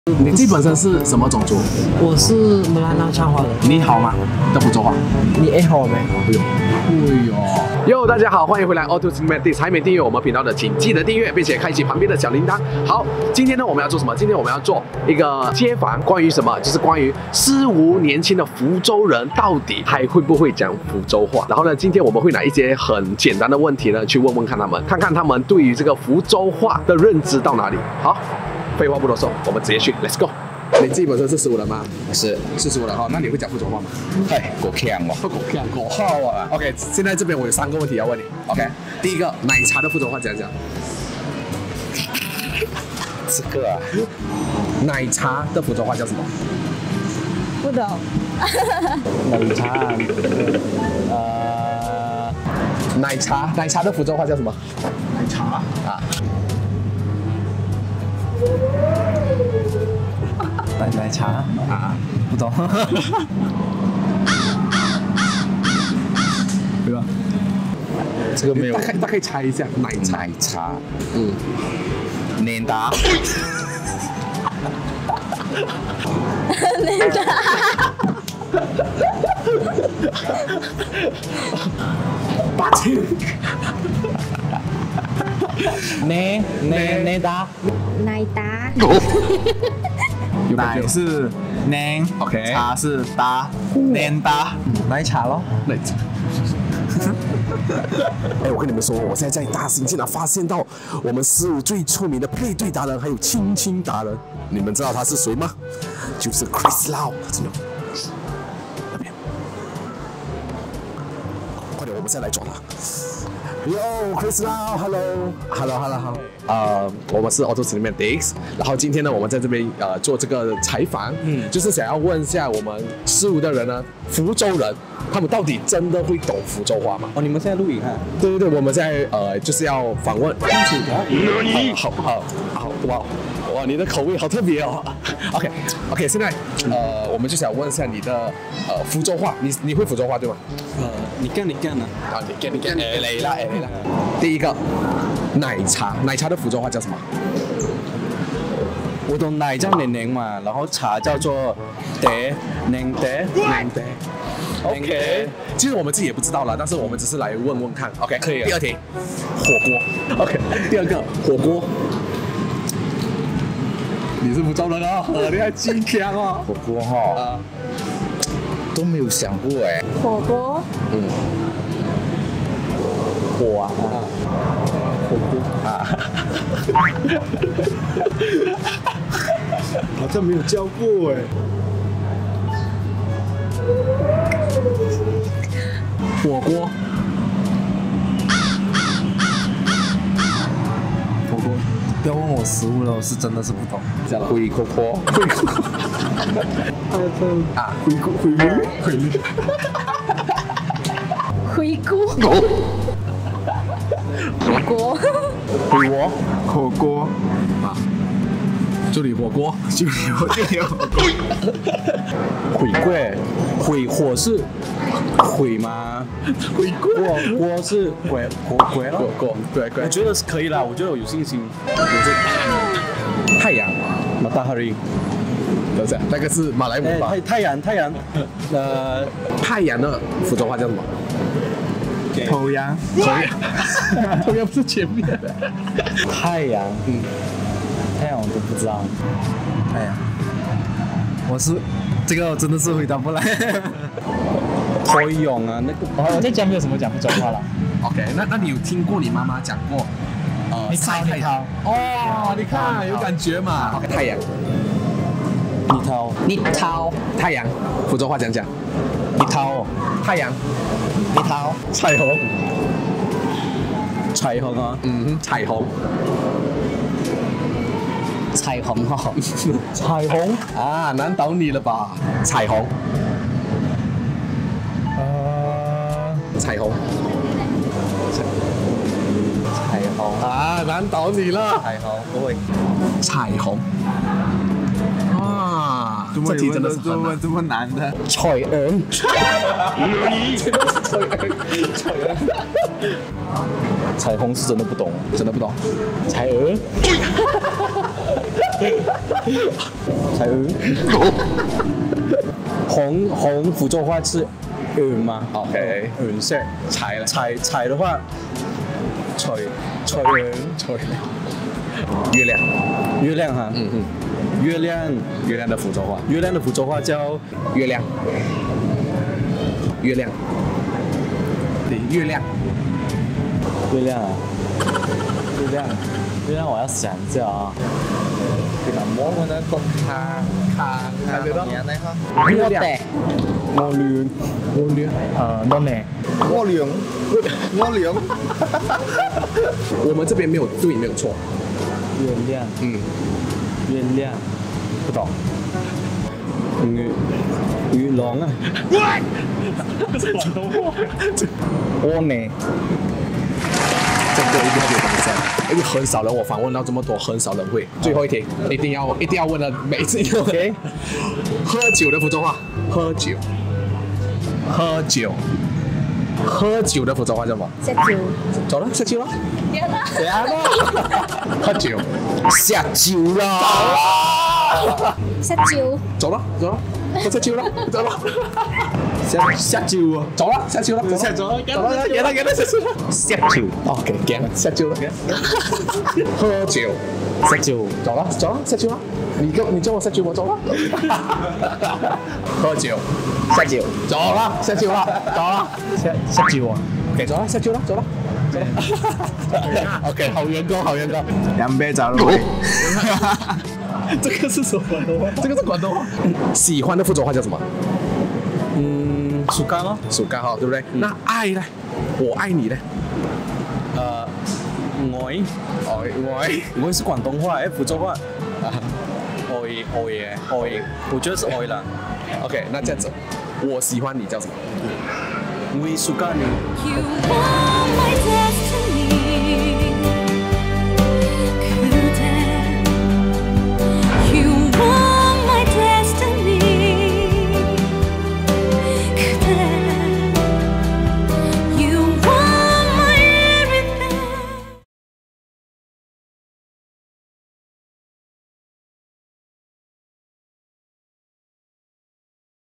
【街訪】難道福州話將要失傳了 ？！
身为诗巫福州人的你还知道什么是福州话吗？
祖语对你来说重要吗？